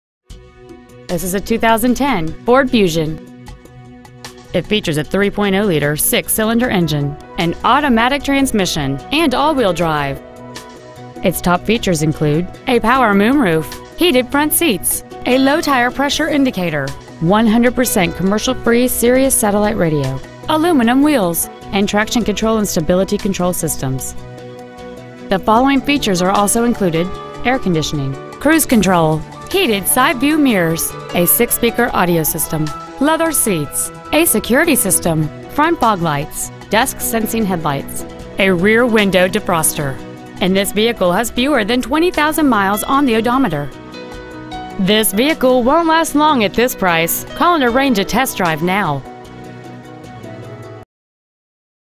2010 Ford Fusion Saugus MA
Year : 2010
 Make : Ford
 Model : Fusion
 Engine : 3.0L V6 24V MPFI DOHC
 Trans . : 6-SPEED AUTOMATIC
 Exterior : TUXEDO BLACK CLEARCOAT METALLIC
 Miles : 19,765
 Interior : CHARCOAL BLACK

 Preowned 2010 Ford Fusion Saugus MA